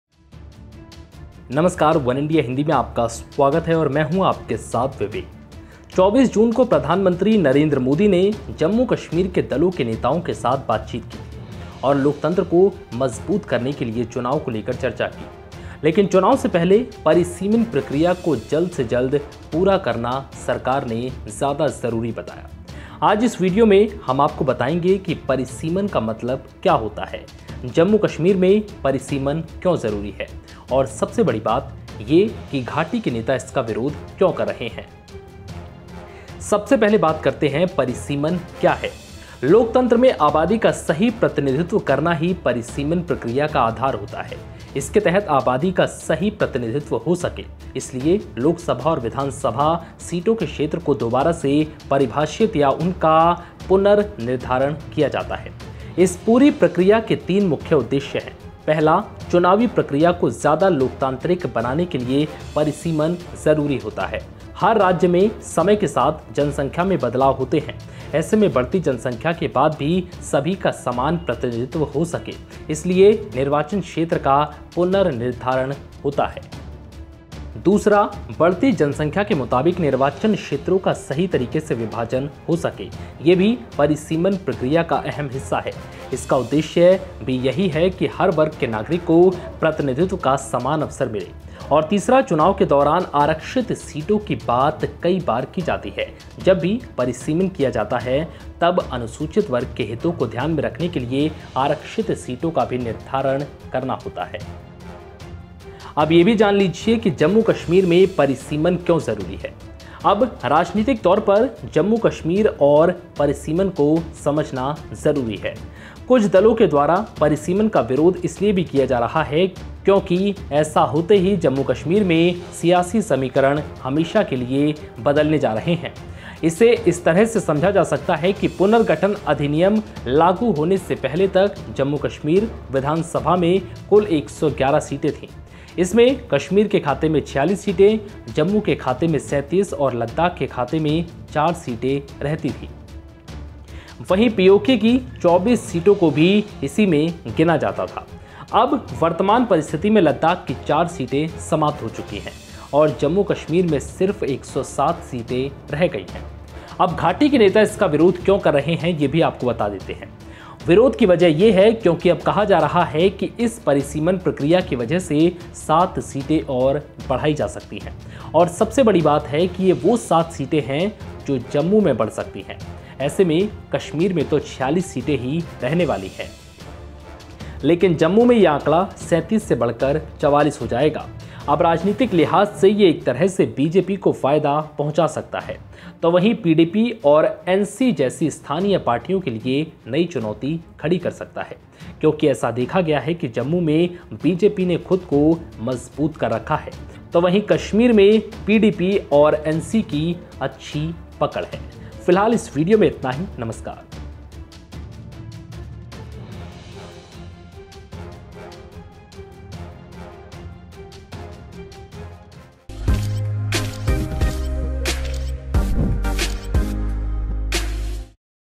परिसीमन क्या है? चुनाव से पहले Jammu Kashmir में ये क्यों जरूरी है? | Delimitation | वनइंडिया हिंदी
Prime Minister Narendra Modi’s meeting with political leaders from Jammu and Kashmir Thursday is being seen as an important prelude to kick-starting political activity in the union territory (UT), which has been suspended since the abrogation of Article 370 in August 2019. The meeting has led to speculation about the possible scheduling of elections in Jammu and Kashmir, thereby necessitating the completion of delimitation in the UT in the next few months. Watch video,

24 जून को PM Modi ने Jammu Kashmir के दलों के नेताओं से बातचीत की थी और लोकतंत्र को मजबूत करने के लिए चुनाव को लेकर चर्चा की. लेकिन चुनाव से पहले परिसीमन प्रक्रिया को जल्द से जल्द पूरा करना सरकार ने ज्यादा जरूरी बताया. आज इस वीडियो में हम आपको बताएंगे कि परिसमीन का मतलब क्या होता है. जम्मू कश्मीर में Delimitation क्यों जरूरी है और सबसे बड़ी बात ये कि घाटी के नेता इसका विरोध क्यों कर रहे हैं. देखिए वीडियो